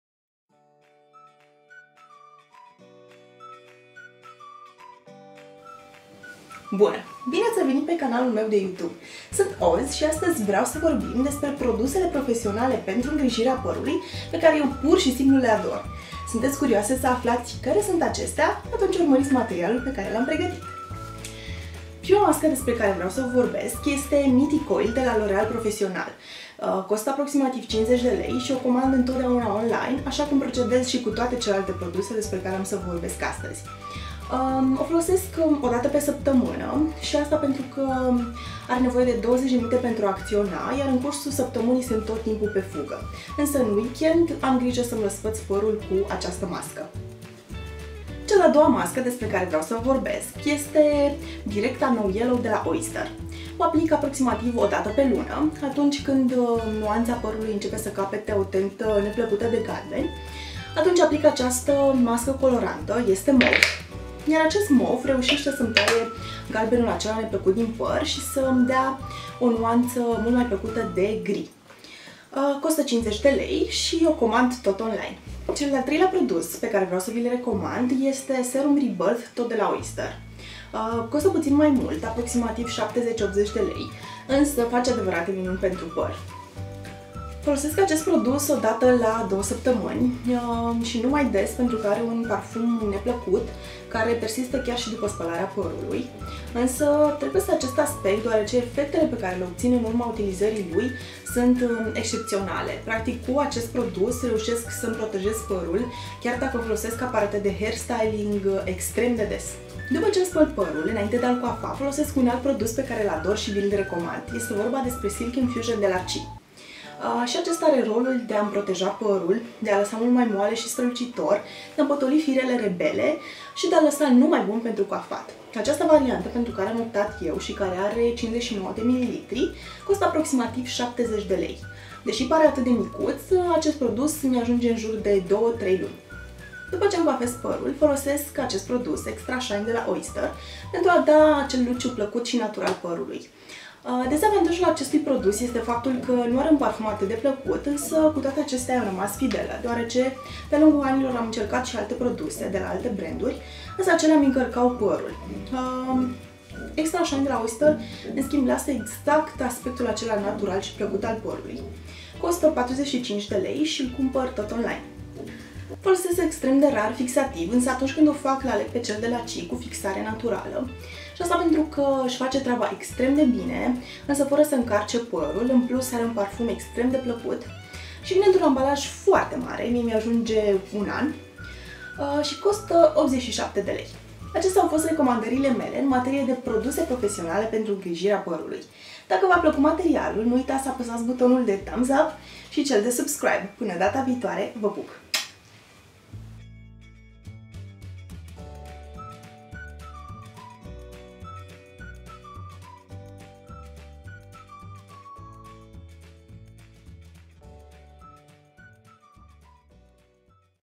Top 6 produse profesionale preferate pentru ingrijirea parului
Cu trecerea timpului, am ajuns sa devin dependenta de anumite produse, foarte bune, fara de care nu mi-as mai putea inchipui ritualul de ingrijjire a parului. Sunt sase la numar si v-am povestit despre ele in acest clip. In postarea de pe blog veti gasi link-urile lor.